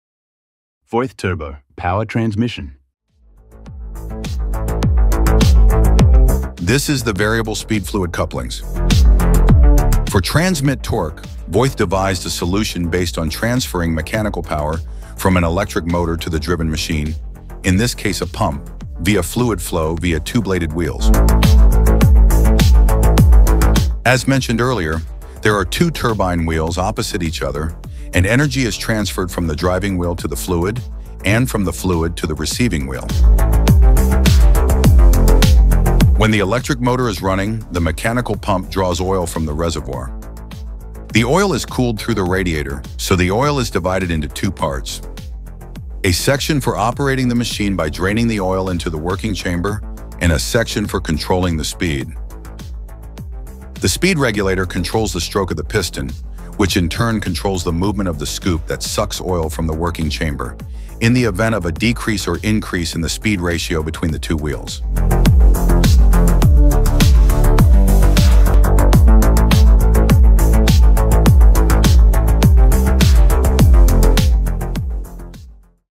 Voith turbo hydrodynamic coupling how's work
In this video, the working principle of the device is demonstrated, transferring mechanical energy from an electric motor to a mechanical device via a hydrodynamic coupling.